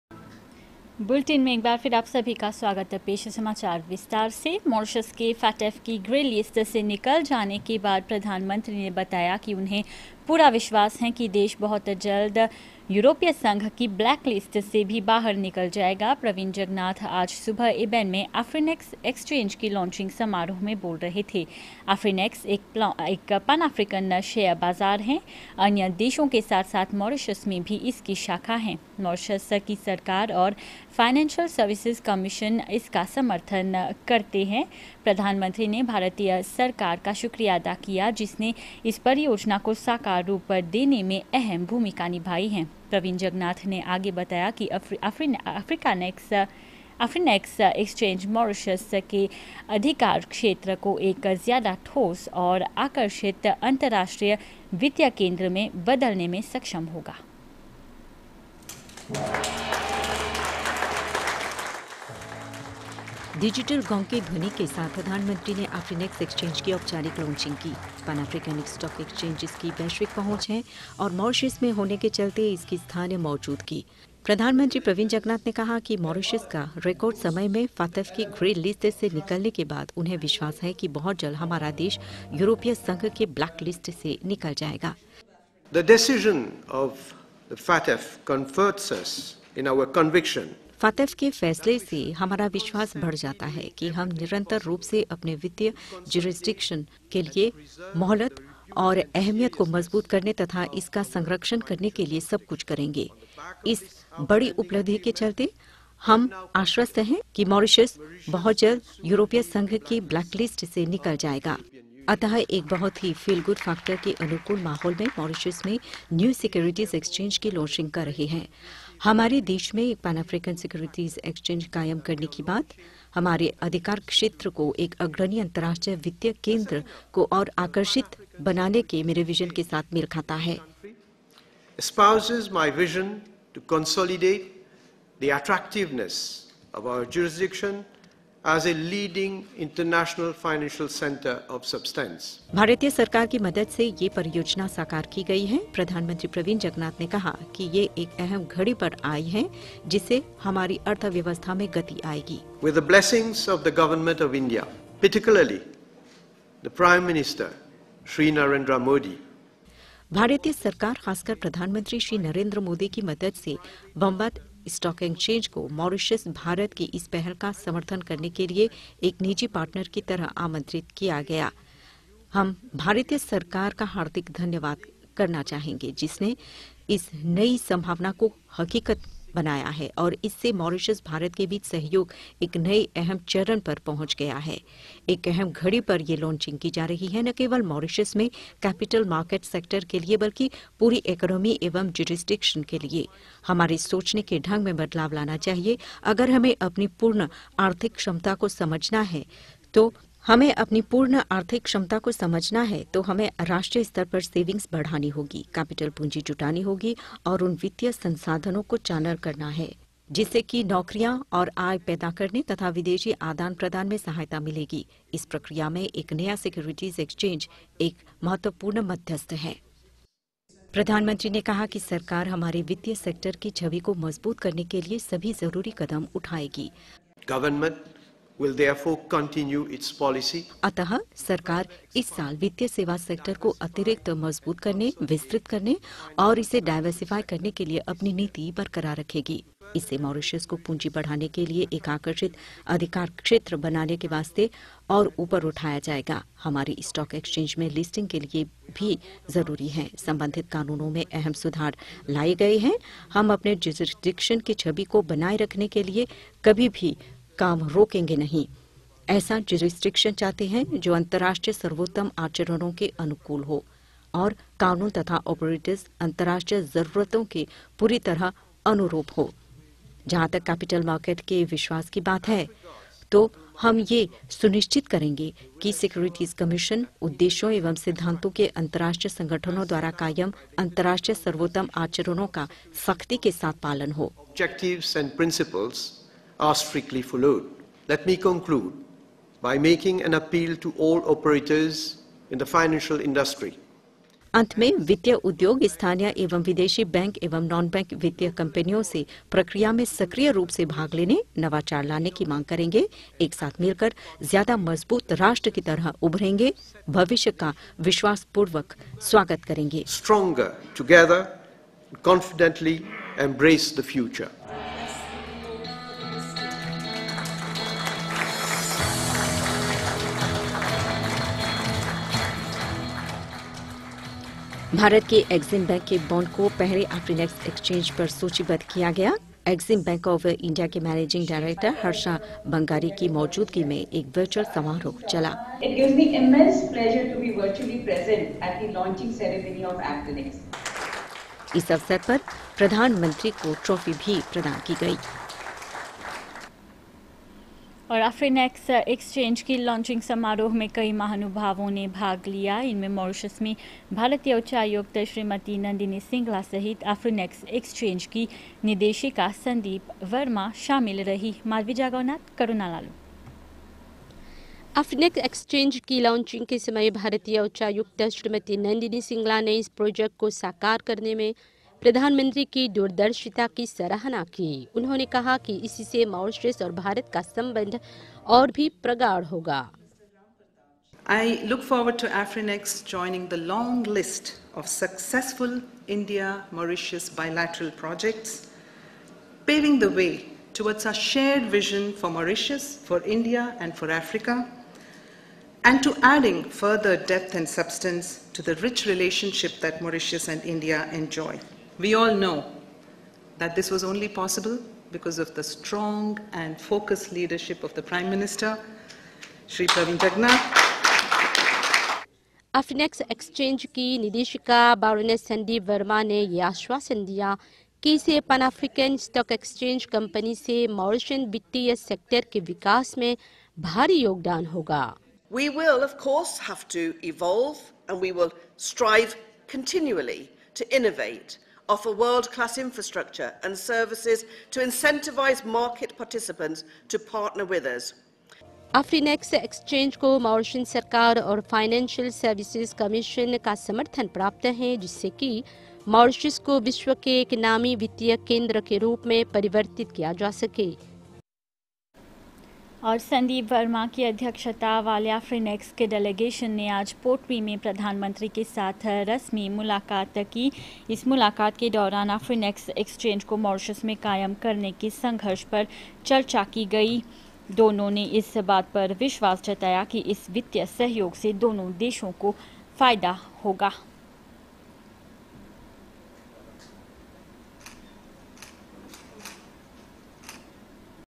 Launch ceremony of AFRINEX | Media Coverage | Samachar 25 October 2021
Launch ceremony of AFRINEX, a pan-African exchange based out of Mauritius, was held today in the presence of Hon. PM Pravind Jugnauth & other eminent personalities.